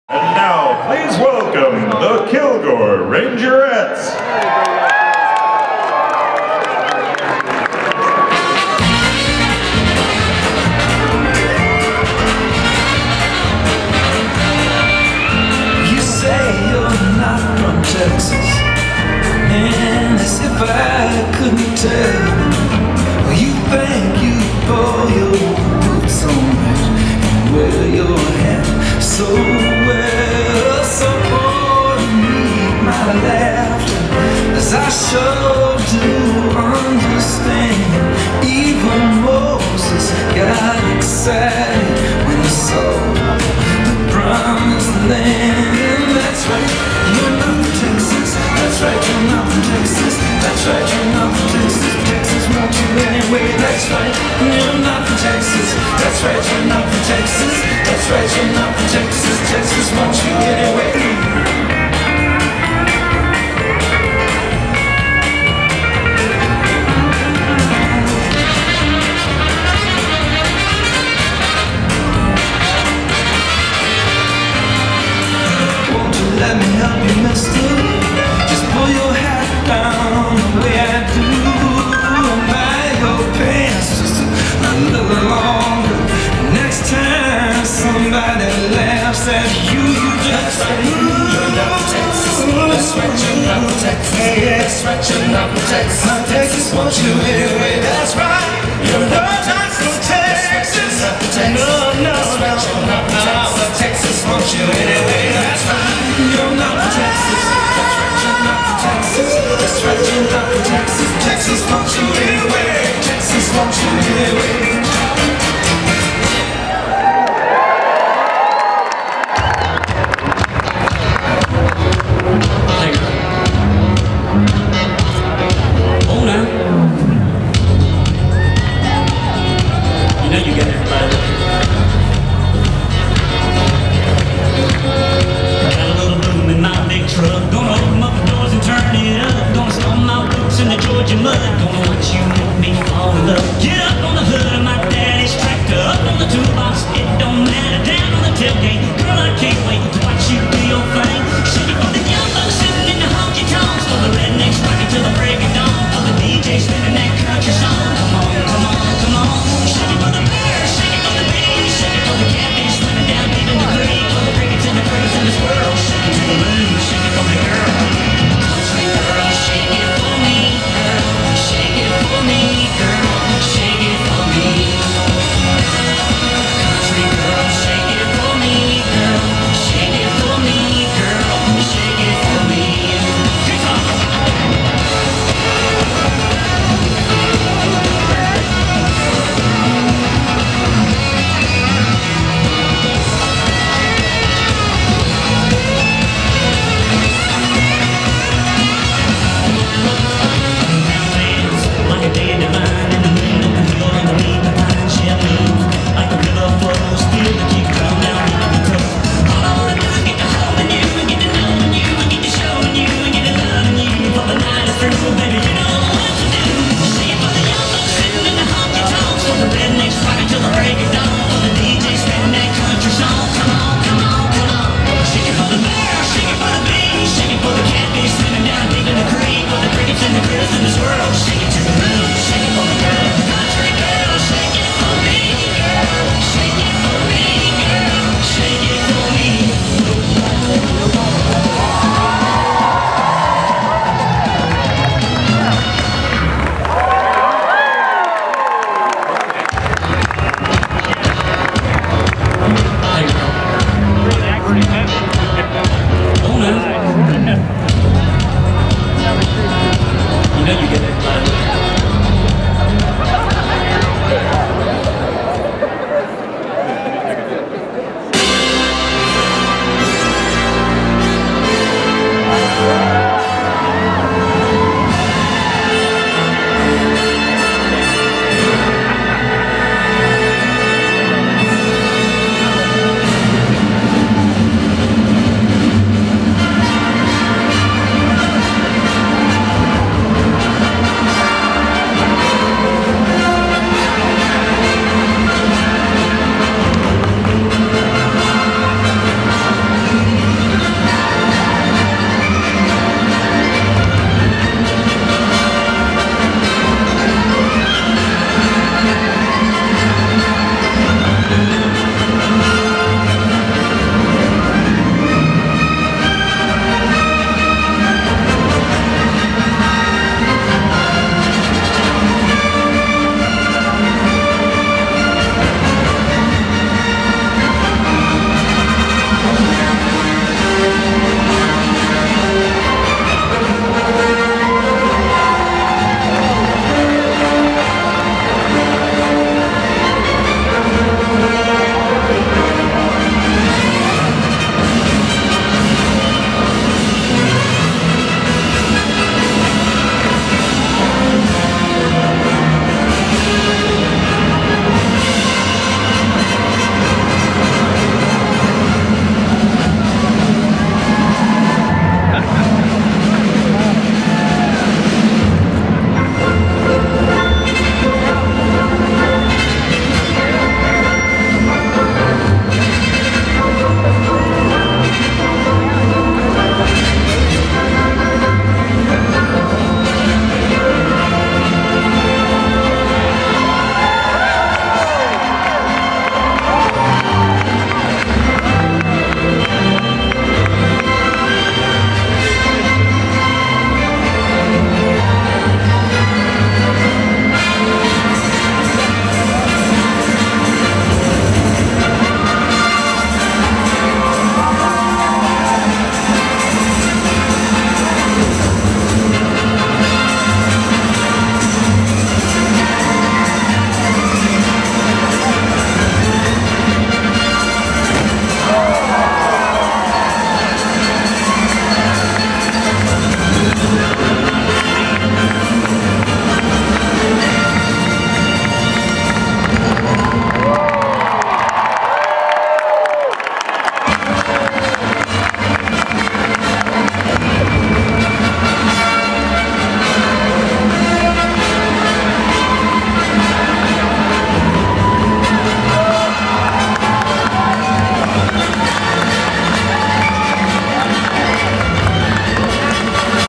Kilgore Rangerettes @ Black Tie & Boots Ball 2013 - 2nd Performance
The Kilgore Rangerettes performed to big crowds in two different ballrooms at the Texas State Society Black Tie & Boots Ball. This was the early performance.